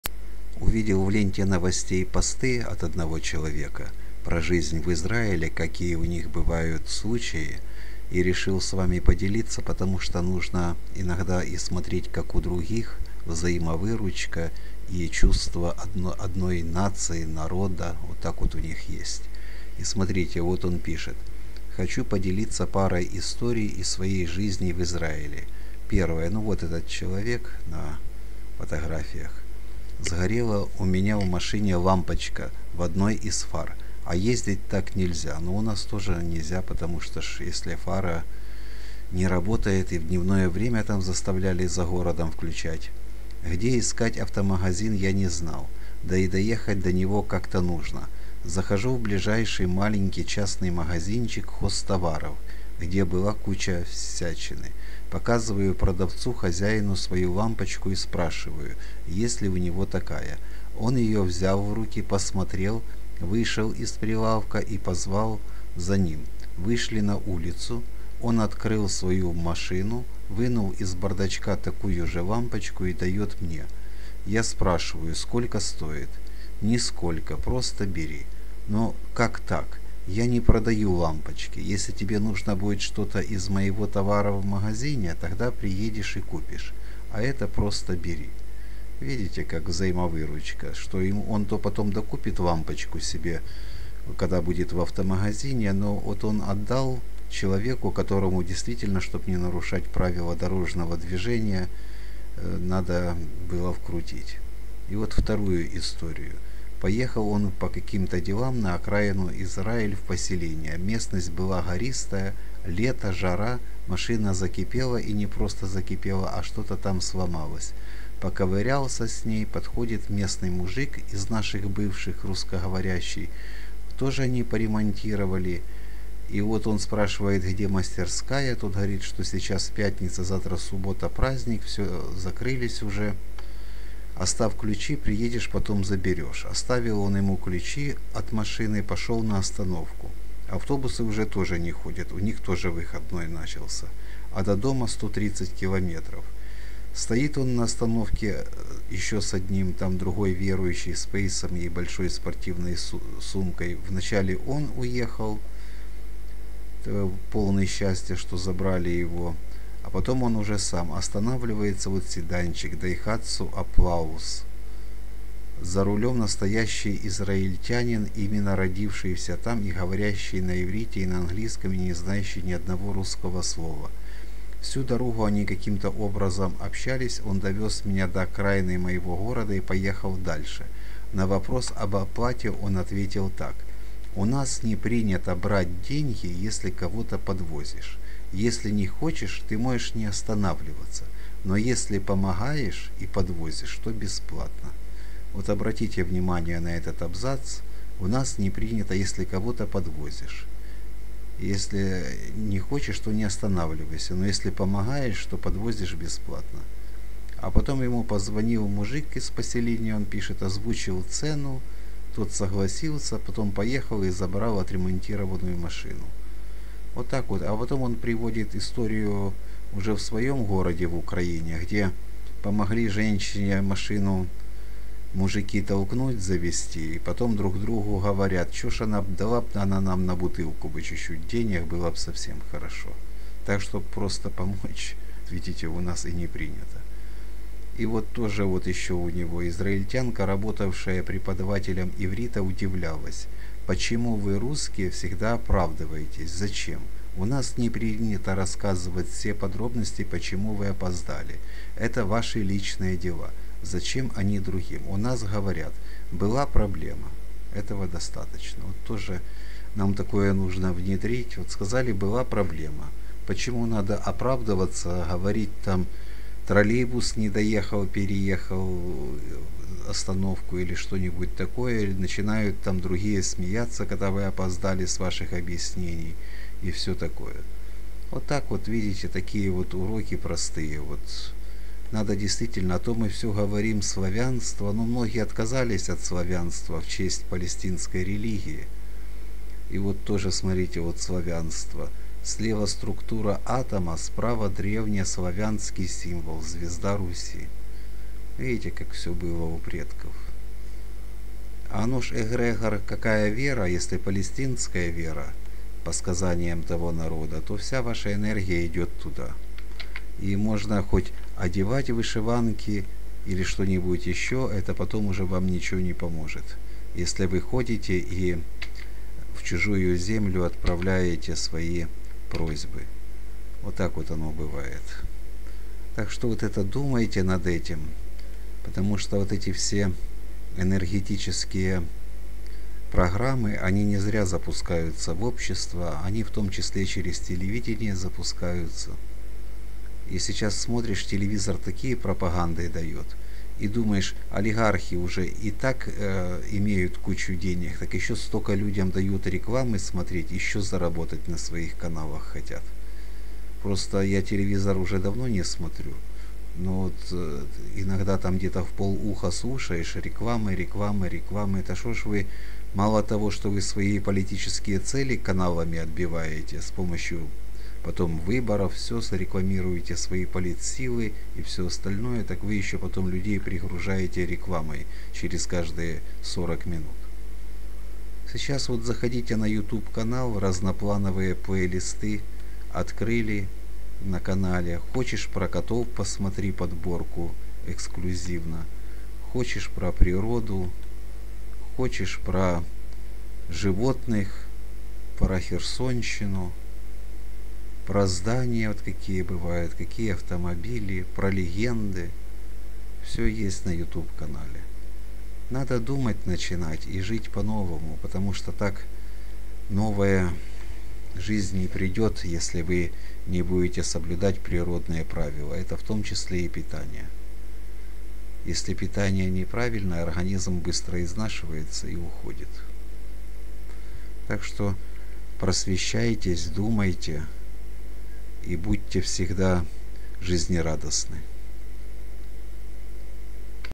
Три урока из Израиля, отношения в обществе и общность людей. Случаи их жизни. Взаимовыручка
Есть ещё, чудесный маленький рассказ у Вл. Богомолова - "Везде люди" называется!

Ведь правда, Везде есть добрые Люди!!!